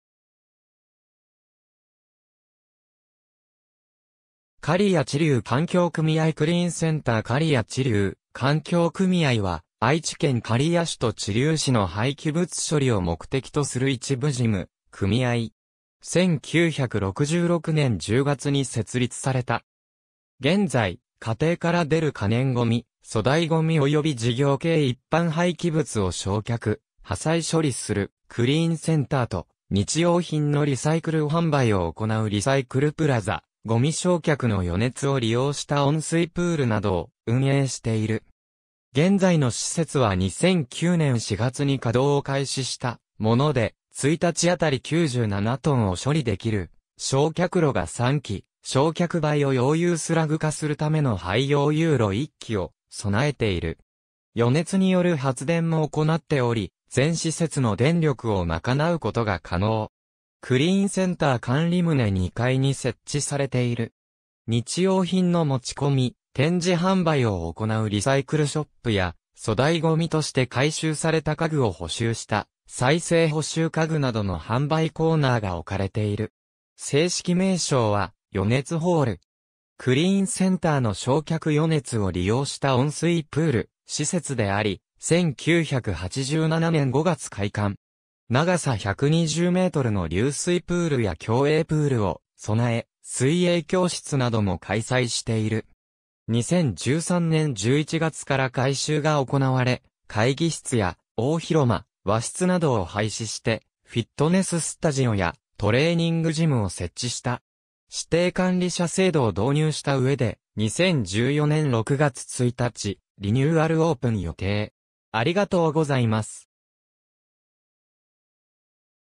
刈谷知立環境組合
刈谷知立環境組合クリーンセンター 刈谷知立環境組合（かりやちりゅうかんきょうくみあい）は愛知県刈谷市と知立市の廃棄物処理を目的とする一部事務組合。
1966年（昭和41年）10月に設立された。
現在、家庭から出る可燃ゴミ・粗大ゴミおよび事業系一般廃棄物を焼却・破砕処理するクリーンセンターと、日用品のリサイクル販売を行うリサイクルプラザ、ゴミ焼却の余熱を利用した温水プールなどを運営している。
現在の施設は2009年（平成21年）4月に稼動を開始したもので、1日あたり97トンを処理できる焼却炉が3基（計291トン/日）、焼却灰を溶融スラグ化するための灰溶融炉1基（20トン/日）を備えている。
予熱による発電（6,400キロワット）も行っており、全施設の電力をまかなうことが可能。
クリーンセンター管理棟2階に設置されている。
日用品の持ち込み・展示販売を行うリサイクルショップや、粗大ゴミとして回収された家具を補修した再生補修家具などの販売コーナーが置かれている。
正式名称は余熱ホール。
クリーンセンターの焼却余熱を利用した温水プール施設であり、1987年（昭和62年）5月開館。
長さ120メートルの流水プールや競泳プールを備え、水泳教室なども開催している。
2013年（平成25年）11月から改修が行われ、会議室や大広間、和室などを廃止してフィットネススタジオやトレーニングジムを設置した。
指定管理者制度を導入した上で、2014年（平成26年）6月1日リニューアルオープン予定。